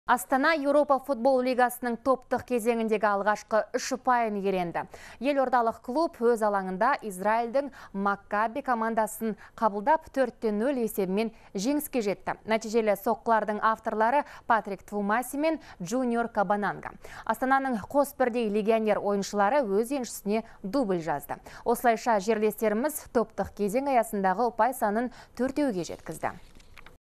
Еуропа лигасы: «Астана» алғашқы үш ұпайына қол жеткізді
«Астана» Еуропа лигасының топтық кезеңіндегі алғашқы үш ұпайын иеленді.